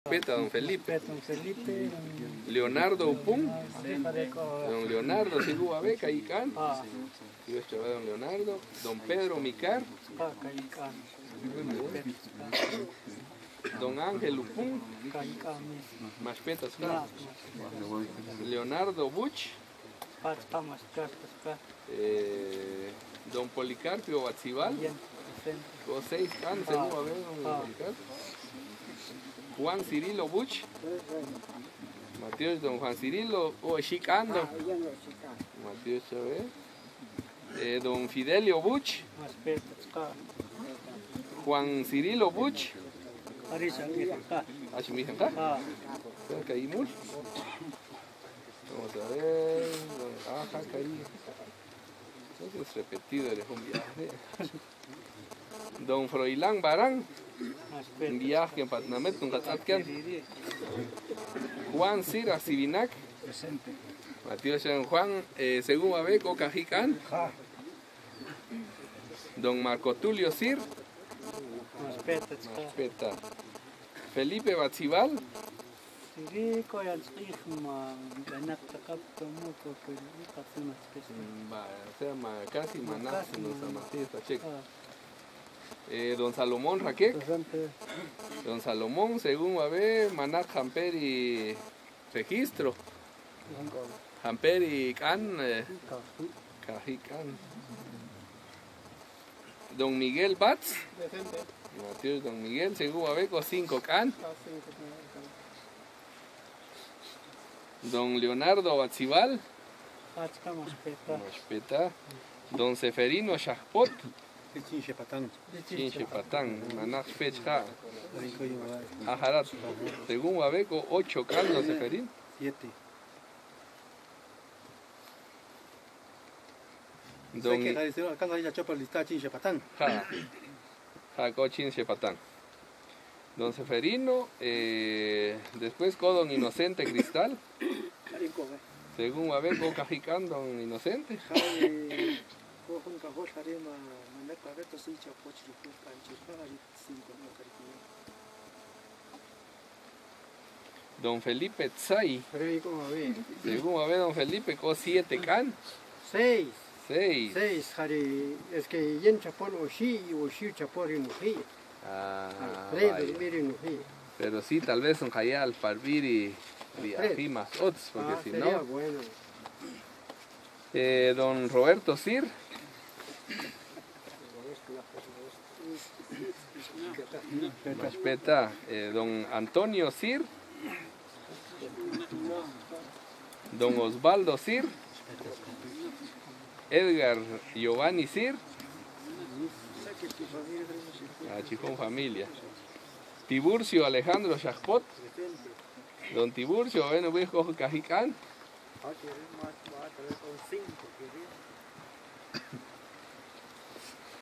Toma de asistencia por parte del Alcalde Municipal
Patzún, el alcalde Municipal Reyes Patal Yos, confirma datos de usuarios en el astillero.